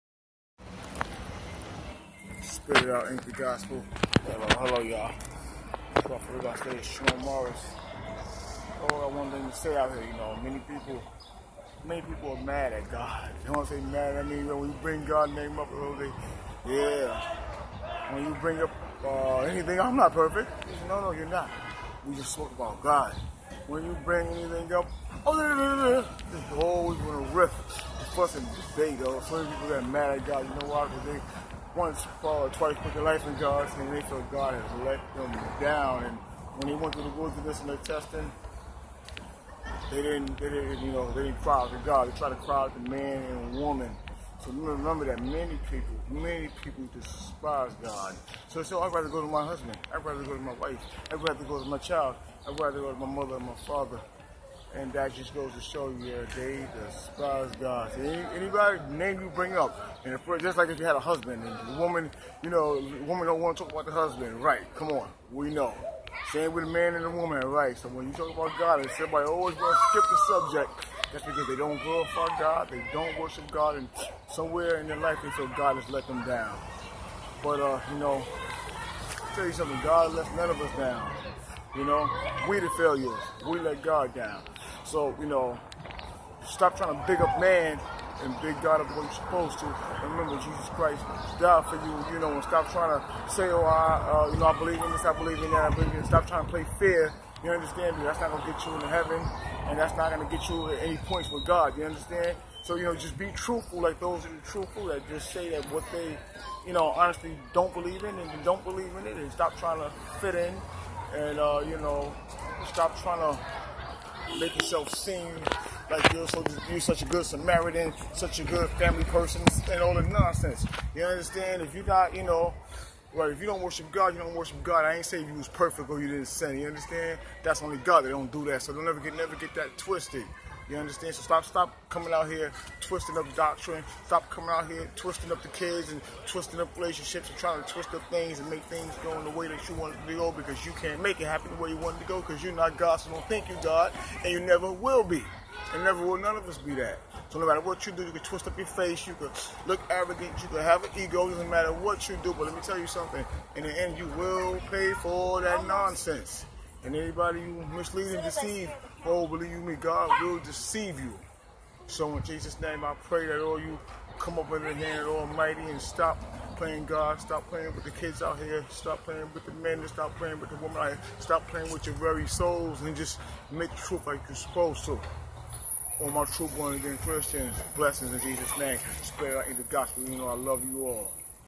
spit it out inc, the gospel: they despise GOD
THEY DESPISE GOD, IT'S SO SAD THEY CRY TO THEIR HUSBANDS WIVES MOTHERS FATHERS FRIENDS PASTORS, AND SO ON, JESUS WAS NEVER THEIR HIGH PRIEST THEY REJECTED HIM EVERY TIME U BRING UP GOD NAME THEY MISTAKE U MARTIN LUTHER KING MALCOLM GHANDI, A PSYCHIC, A NARC A PASTOR OR SOMEONE ELSE FOR GOD BRING UP GOD'S NAME WATCH THEIR FACE AND THE COMMENTS, BUT WE LOVE GOD AND IS OBVIOUS WE GOT ENOUGH LOVE IN US TO LOVE GOD FOR THOSE THAT HATE HIM, IN JESUS NAME WE LOVE U GOD